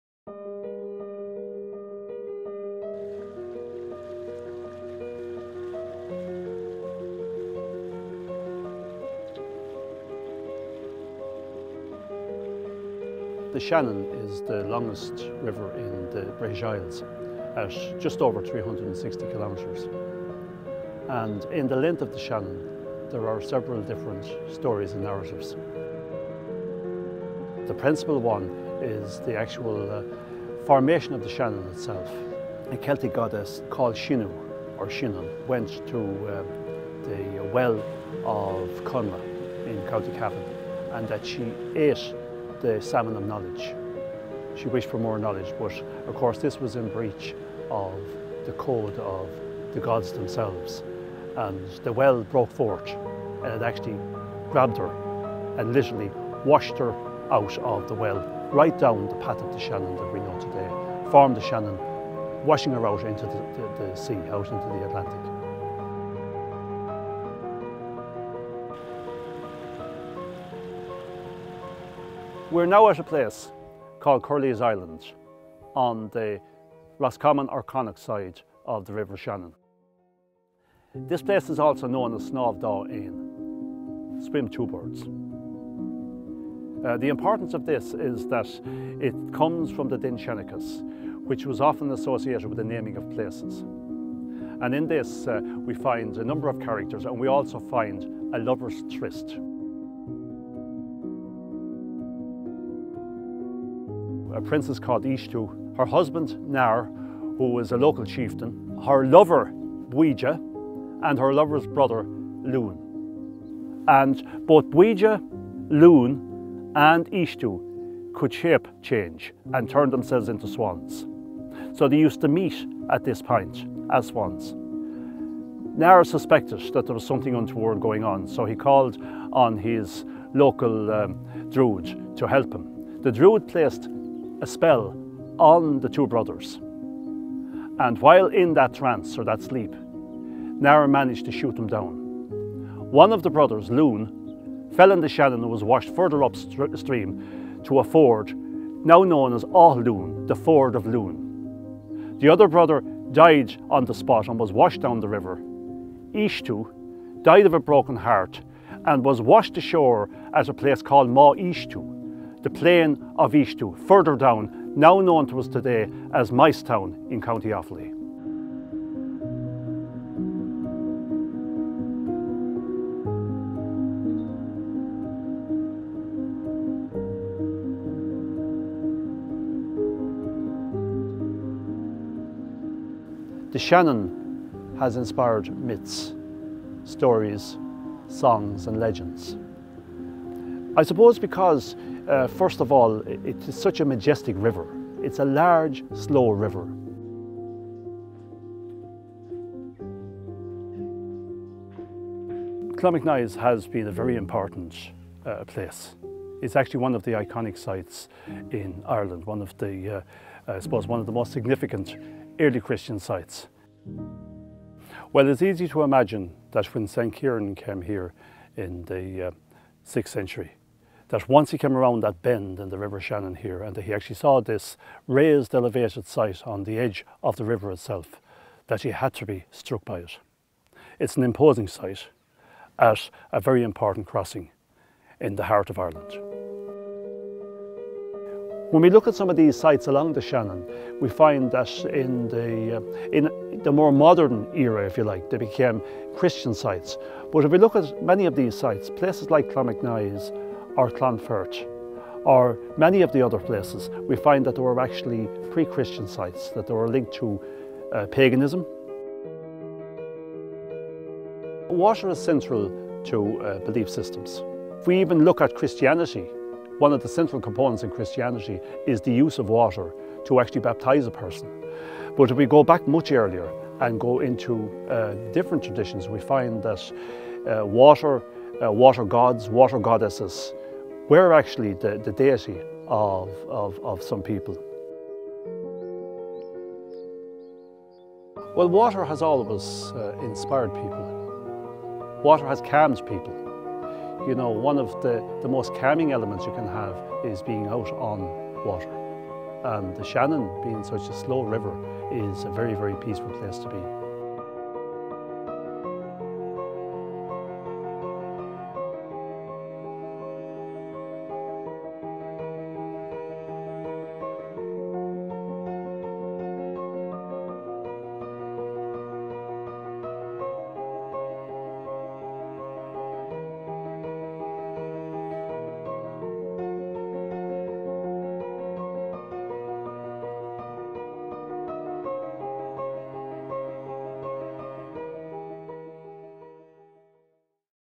Stories from the Waterways - The Spirit of the Shannon
Delighted to premier the first of the 2022 Stories from the Waterways film series, an initiative of the Waterways Ireland Heritage Plan. This year we focus on how our shared inland waterways inspire individuals and groups.  In this film we join Christy Cunniffe as he explains how early religions, spirituality and folklore were inspired by the Shannon. To this day we are calmed by being around water.  Many thanks to Robbie Bayly of the Heritage Boat Association and OPW Clonmacnoise for their assistance.